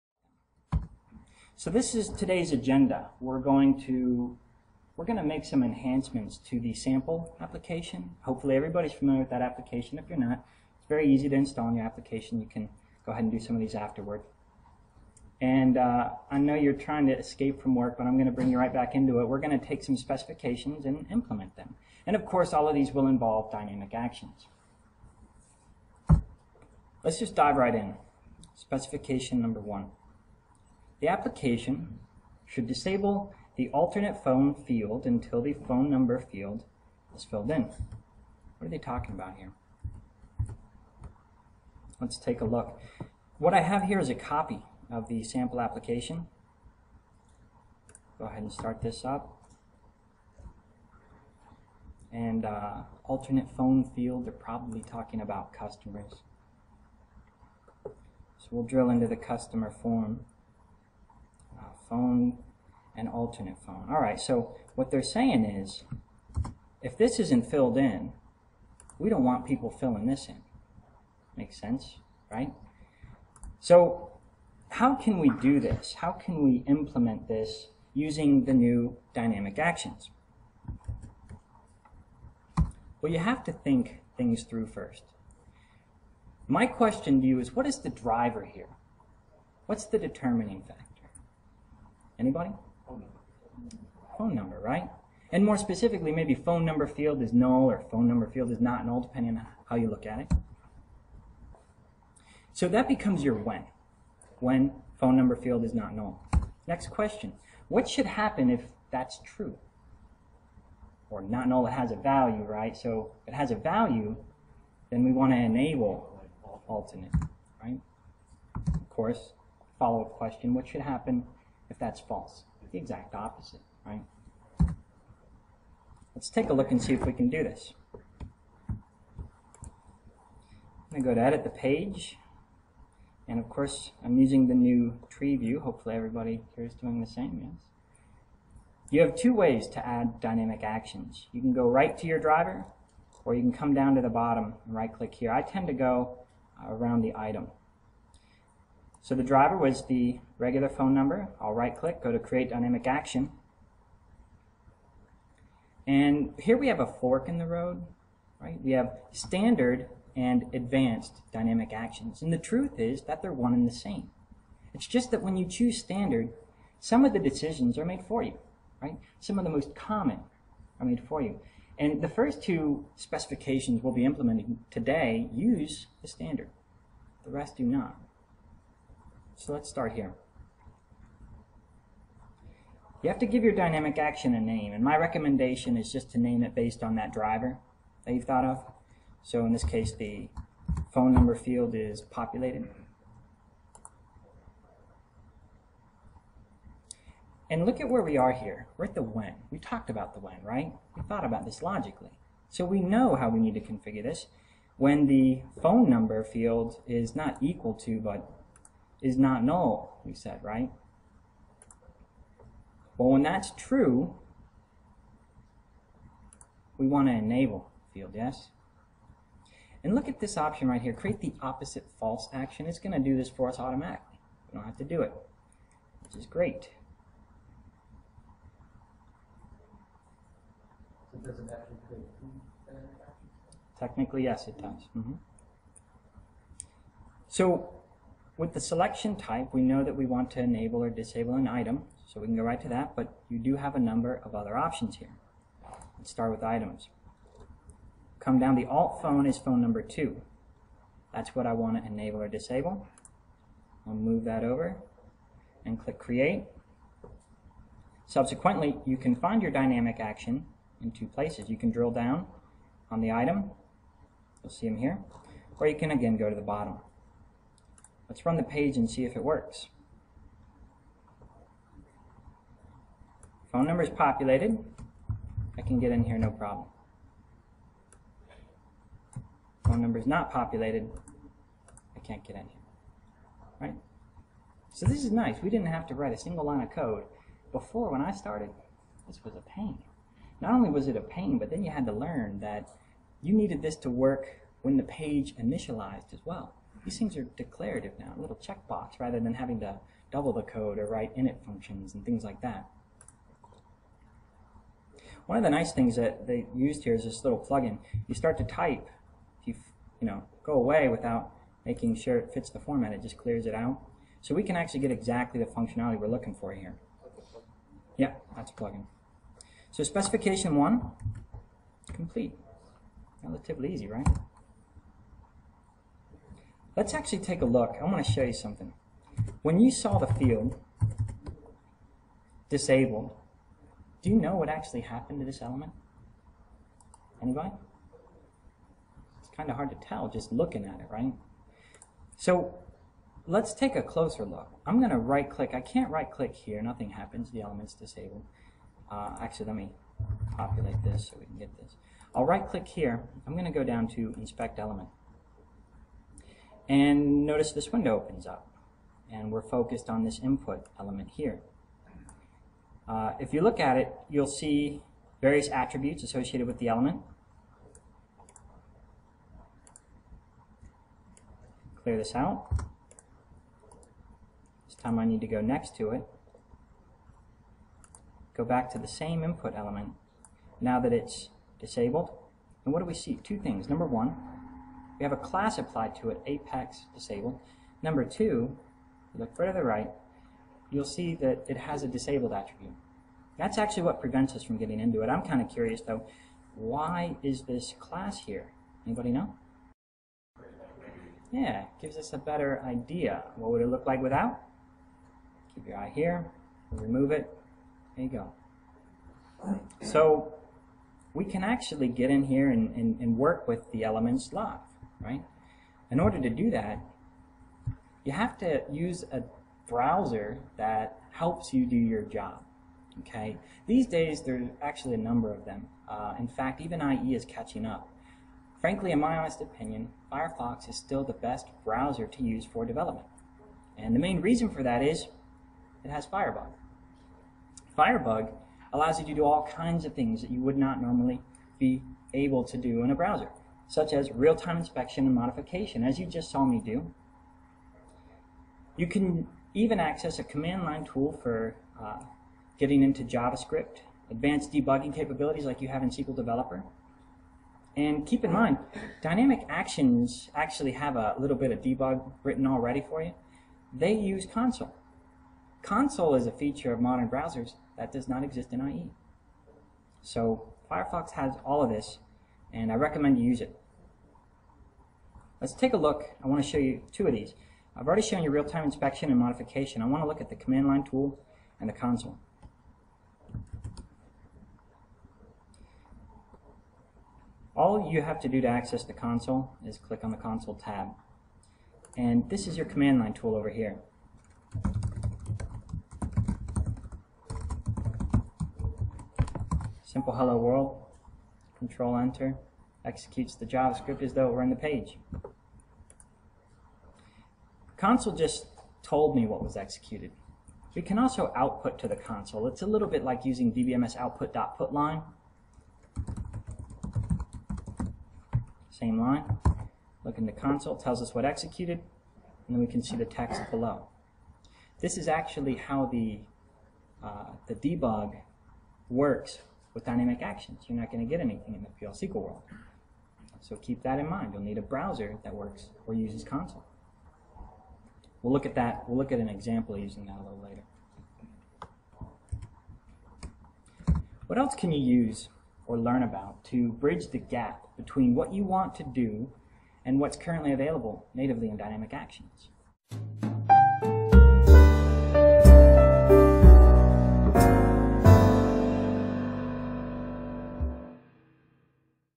Oracle APEX 4.0 Dynamic Actions Tutorial
SkillBuilders APEX expert Dan McGhan presents at ODTUG APEXPOSED 2010 in Dallas: "Dynamic Actions in APEX 4.0: What's Possible, What's Not and How to Bridge the Gap." Dan explores the declarative side of Dynamic Actions to find out what's possible and what's not, and demonstrates what you need to know to bridge the gap.